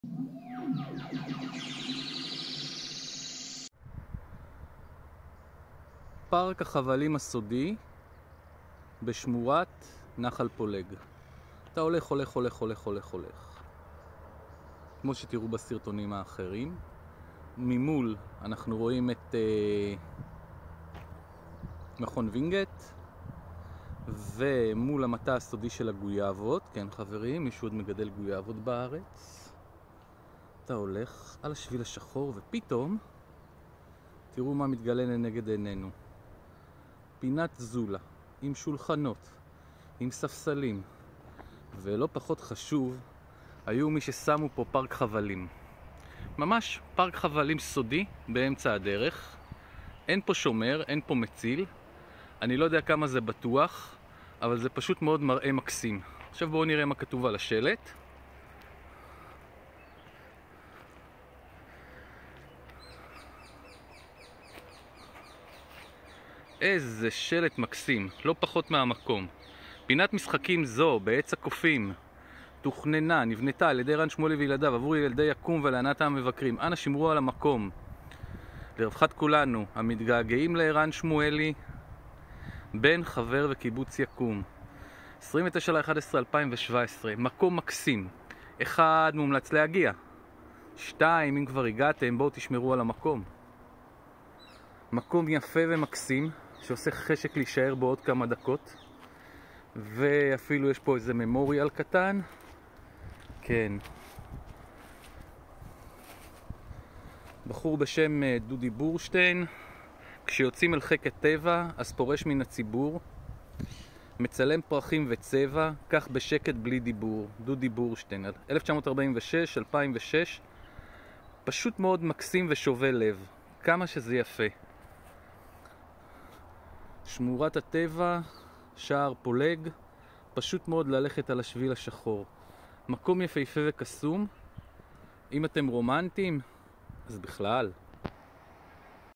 שער נחל פולג + פארק חבלים טיולים מומלצים דרגת קושי קלה מסלול מעגלי מתאים לכל עונות השנה לאן מטיילים
שער נחל פולג טיולים מומלצים דרגת קושי קלה מסלול מעגלי מתאים לכל עונות השנה לאן מטיילים בשבת
במקום תוכלו למצוא פארק חבלים שהוקם על ידי ערן שמואלי חבר קיבוץ יקום להנאת ילדי יקום והמטיילים. אחת מפינות החמד היפות במרכז הארץ. במקום גם מצבת זיכרון לזכרו של דודי בורשטיין. מקום קסום ורומנטי.

אם הגעתם לטייל במקום - בבקשה - שמרו על הניקיון, מומלץ להביא שקית קטנה לאשפה ולקחת אתכם. בנוסף קחו בקבוק מים וטלפון סלולארי.

זמן הטיול במקום עד שעתיים אבל אפשר פחות או יותר. מתאים בתור טיול לכל המשפחה במרכז. עשר דקות נסיעה מתל אביב.

איך מגיעים  ? על כביש תל אביב חיפה פונים שמאלה בפארק יקום עד לנקודת הכניסה לשמורה.

דמי כניסה  - אין.
אגב זה אחד המקומות הרומנטיים באזור גוש דן. נגיש, קליל ובעיקר עושה טוב על הלב. על השבילים חולפים מעת לעת רוכבי אופניים.

טיול נעים